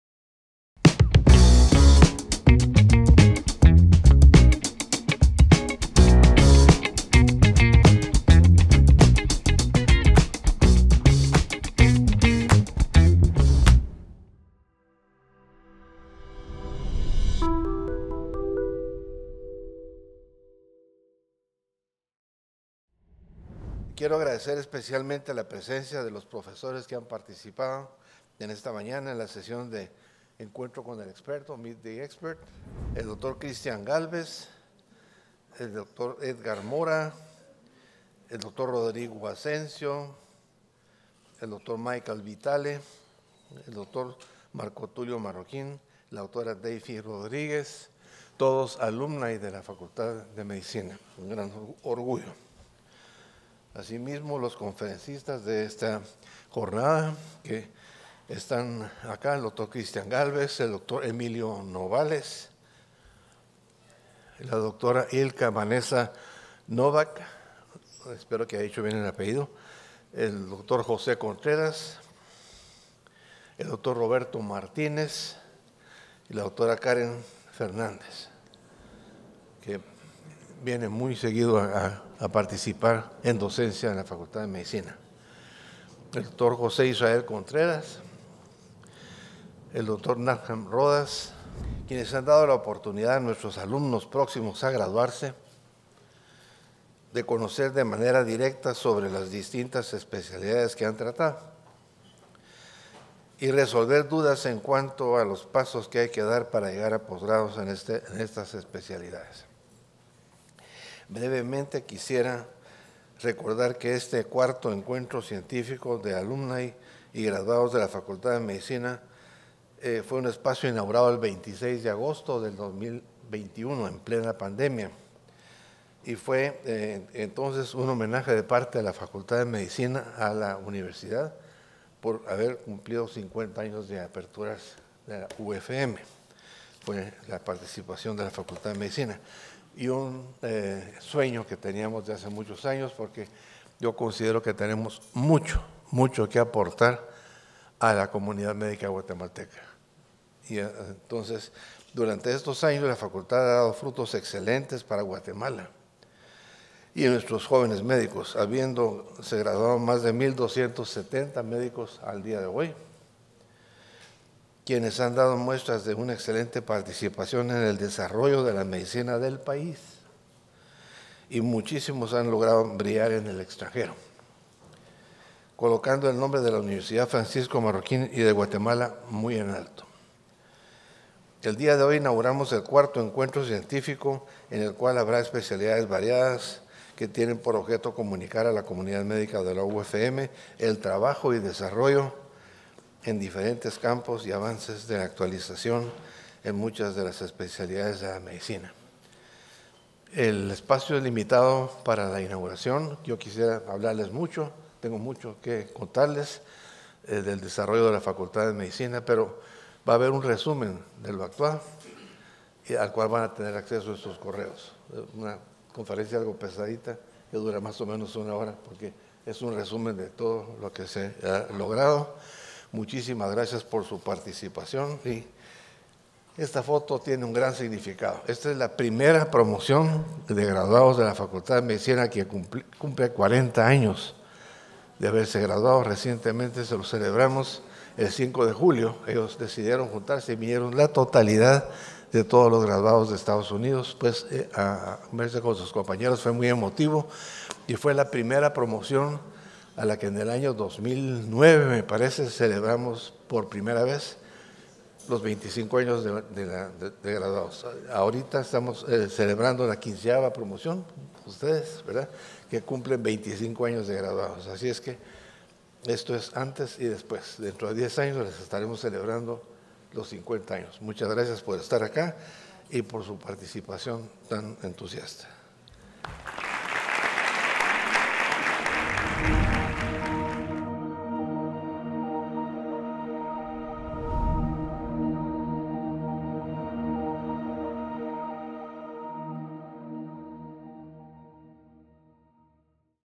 Bienvenida por el decano Federico Alfaro
El Cuarto encuentro científico de la Facultad de Medicina de la Universidad Francisco Marroquín resalta la importancia de la medicina desde un enfoque integral.

En esta edición, líderes de la salud y egresados de la UFM comparten sus conocimientos a través de conferencias educativas, presentando investigaciones médicas, logros y retos que han enfrentado en su trayectoria profesional. Este evento no solo es una oportunidad para aprender de expertos, sino también para reflexionar sobre la evolución de la medicina y su impacto en el bienestar humano.

Te invitamos a ver el Tercer encuentro científico de medicina 2023

Organizado por:
Facultad de Medicina